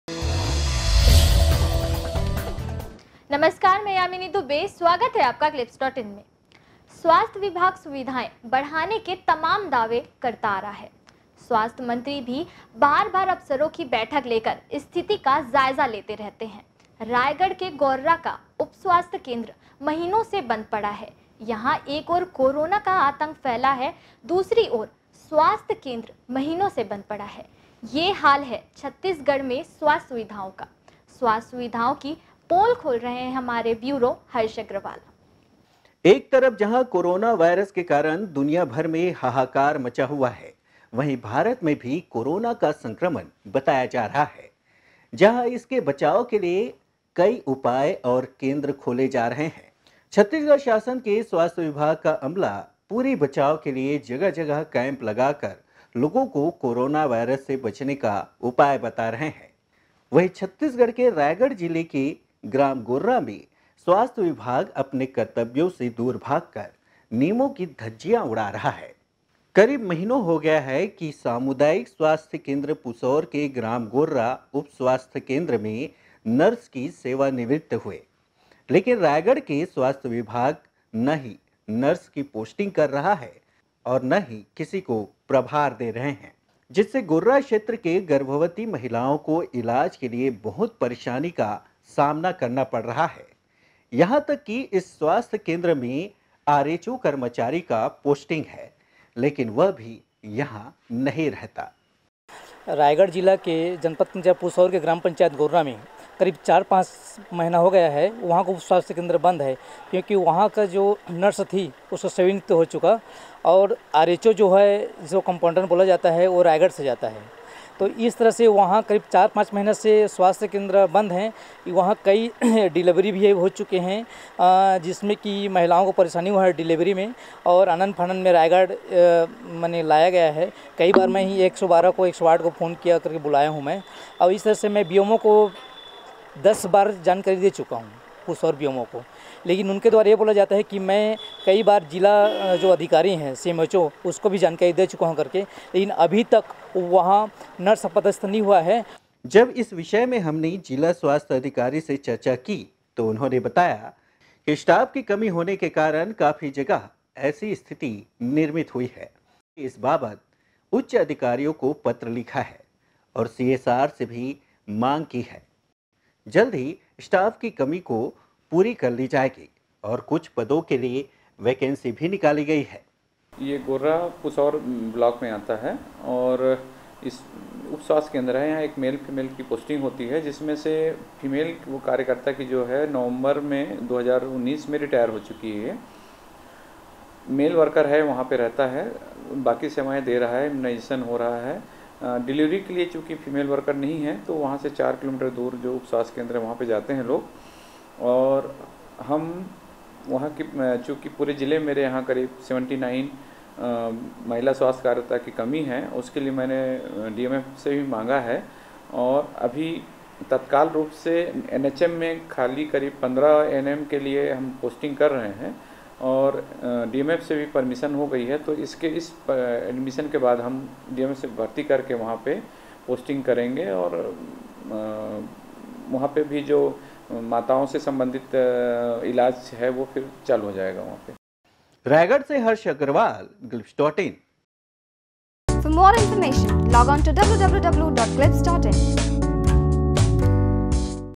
महीनों से बंद है गोर्रा का उपस्वास्थ्य केंद्र, इलाज के लिए भटक रहे मरीज
रायगढ़। एक तरफ जहां कोरोना वायरस का दुनिया में हाहाकार मचा हुआ है। भारत मे भी कोरोना का संक्रमण बताया जा रहा है। जहाँ इसके बचाव के लिये  उपाय और केंद्र व छत्तीसगढ़ शासन के स्वास्थ्य विभाग का अमला पूरी बचाव के लिये जगह-जगह कैम्प लगाकर लोगों को कोरोना वायरस से बचने का उपाय बताया जा रहा है। वहीं रायगढ़ जिले के ग्राम गोर्रा में स्वास्थ्य विभाग नियमों का धज्जियाँ उड़ा रहा है। कुछ महीनों पहले स्वास्थ्य केंद्र पुसौर के ग्राम गोर्रा में उपस्वास्थ्य केंद्र की नर्स सेवा निवृत्त हो गई। लेकिन रायगढ़ के स्वास्थ्य विभाग न ही नर्स की पोस्टिंग कर रहा है और न ही अस्पताल को किसी प्रभार में नही दिया जा रहा है। जहाँ क्षेत्र के मरीजों को इलाज के लिए परेशानी का सामना करते हुए भटकना पड़ रहा है। यहाँ तक के उक्त स्वास्थ्य केंद्र में आर एच ओ कर्मचारी की पोस्टिंग है लेकिन वह भी स्वास्थ्य केंद्र में नही रहता है। जब इस विषय पर जिला स्वास्थ्य अधिकारी से चर्चा की गई तो उन्होंने बताया स्टाफ की कमी होने के कारण काफी जगह ऐसी स्थिति निर्मित हुई है। हमने इस बाबत उच्च अधिकारियों को पत्र लिखा है और सीएसआर से भी मांग की है। जल्द ही स्टाफ कि कमी को पूरी कर ली जाएगी और कुछ पदों के लिए वैकेंसी भी निकाली गई है।